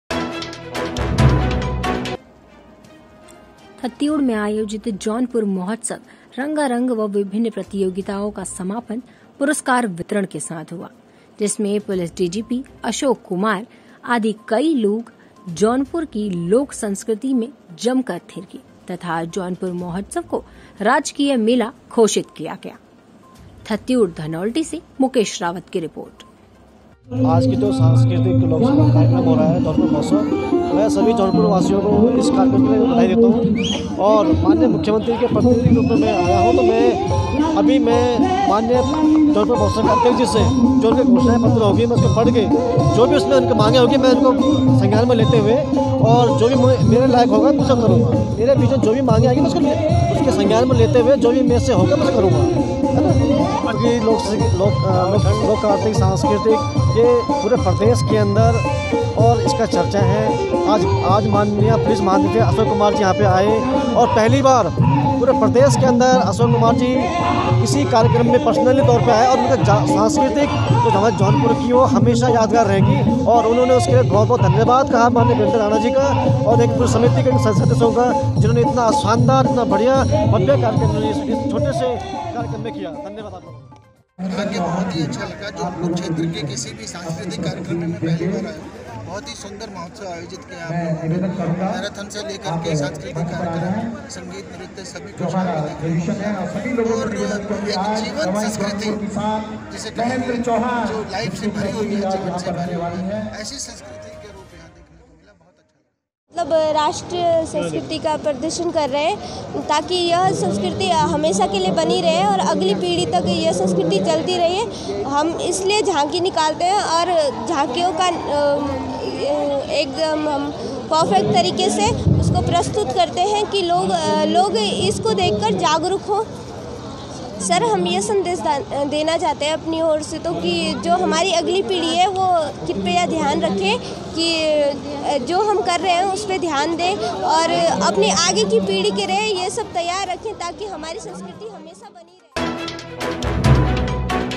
थत्यूड़ : जौनपुर महोत्सव में अशोक कुमार डीजीपी गढवाल जौनपुर की लोक संस्कृति में जमकर थिरके
टॉप-थत्यूड़/धनोल्टी
संवाददाता-मुकेश रावत
स्लग- जौनपुर महोत्सव में अशोक कुमार डीजीपी गढवाल जौनपुर की लोक संस्कृति में  जमकर थिरके

एकर : थत्यूड में आयोजित जौनपुर महोत्सव रंगारगा व विभिन प्रतियोगिताओं का समापन पुरुस्कार वितरण के साथ हुआ। जिसमें पुलिस डीजीपी गढ़वाल अशोक कुमार आदि जौनपुर की लोक संस्कृति में जम कर शिरके। तथा जौनपुर महोत्सव को राजकीय मेला घोषित किया।
विकास खण्ड थत्यूड के राजकीय इंटर कालेज थत्यूड़ के परिसर में आयोजित कार्यक्रम में मैराथन दौड व व्लाक मुख्यालय से मुख्य बाजार होते हुए कार्यक्रम स्थल राइका थत्यूड तक विभिन विद्यालयों के छात्र छात्राओं द्वारा संस्कृति झांकी निकाली। महोत्सव में झांकी प्रतियोगिता,  सामान्य ज्ञान प्रतियोगिता ,गढ़वाली भाषण प्रतियोगिता भी  आयोजित कि गई।
वही रात्री संध्या में बतौर मुख्य अतिथि मुख्यमंत्री के प्रतिनिधि के रूप में व जन सर्पक अधिकारी मुलाम सिंह रावत व पुलिस गढ़वाल डीजीपी  अशोक कुमार द्वारा विभिन प्रतियोगिता के प्रतिभाओं को पुस्कार प्रदान किया ।
संध्या में जौनपुर के सुप्रिद्व गायक कालाकार महेन्द्र चौहान, अकित चौखवाण, गायिका मंजू नौटियाल आदि की सुदर प्रस्तुती पर मध्य रात्री तक समा बांधे रखा।
 जिस पर जौनपुर की संस्कृति पर गढवाल डीजीपी अशोक कुमार सहित अन्य अतिथिगण अपने को रोक नही सके और जमकर ढुमके लगाए ।
वही पूर्व खेल मंत्री नारायण सिह राणा भी अपनी पौराणीक लोक गायक महेन्द्र सिंह चौहान के साथ गीत प्रस्तुत किया ।
मुख्य मंत्री के प्रतिनिधि मुलायम सिह ने जौनपुर महोत्सव को राजकीय मेला घाषित करने के साथ एक लाख रुपये प्रति वर्ष देने की घोषणा कि।